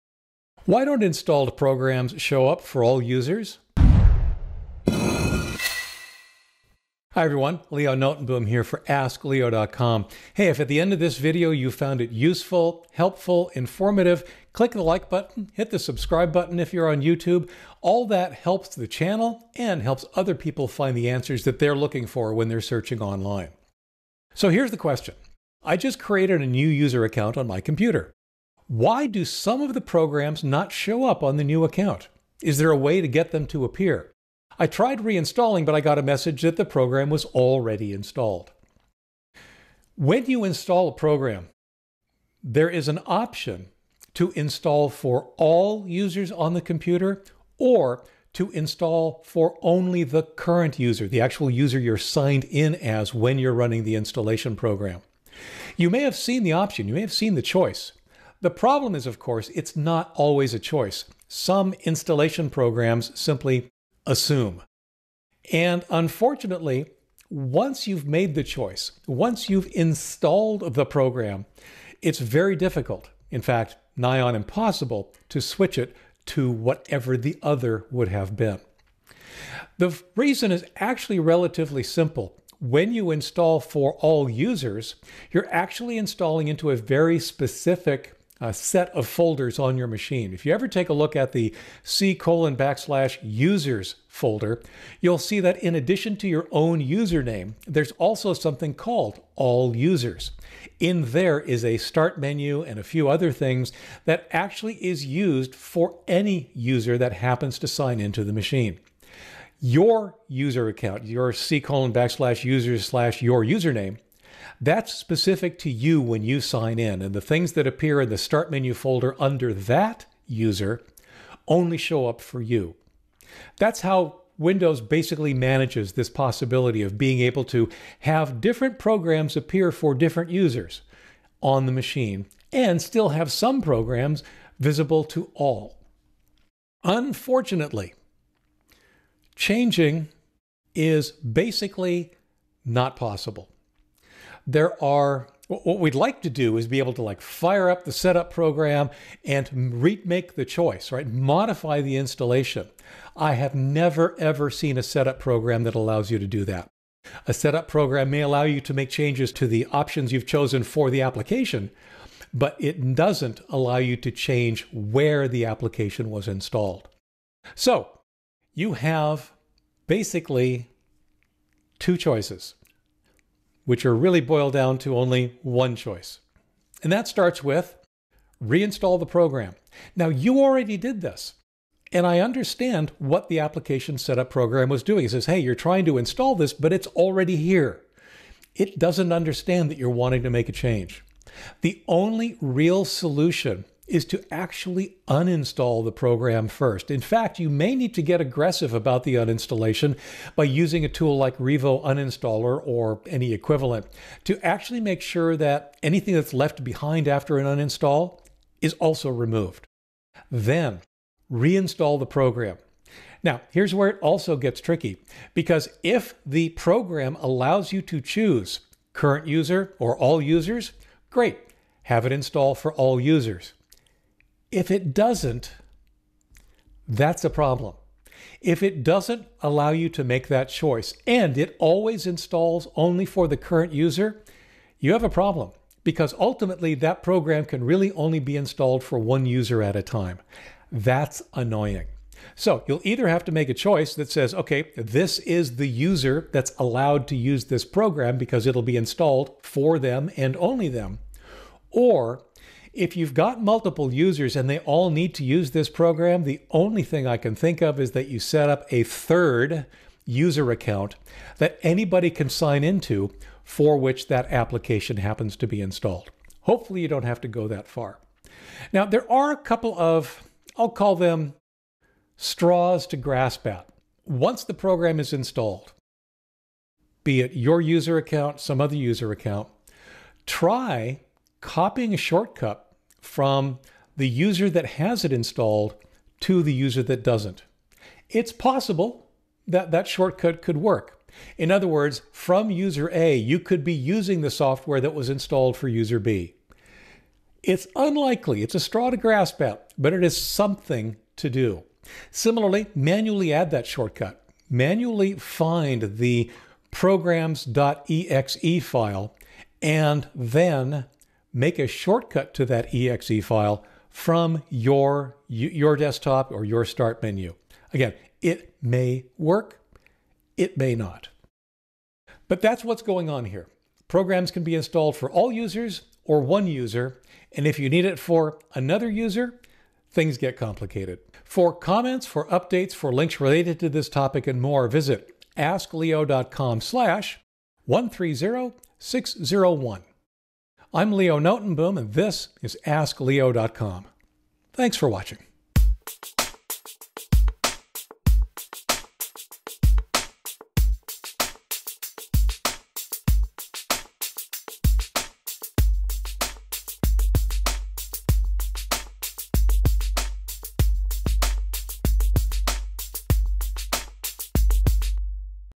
Why Don’t Installed Programs Show Up for All Users?
✅ If an application is installed only for one user, and you want to use it as another, options are limited.

✅ Install for all users?
Many programs allow you to select installation for all users on a computer, or only the current user. Changing that setting after the fact can be difficult. Unless the setup program explicitly accounts for it the most expeditious solution is to uninstall and reinstall with the desired choice.

Chapters
0:00 Installed Programs for All Users
0:30 It's set when you install
2:15 Changing it
2:55 Uninstall the program
4:30 Straws to grasp at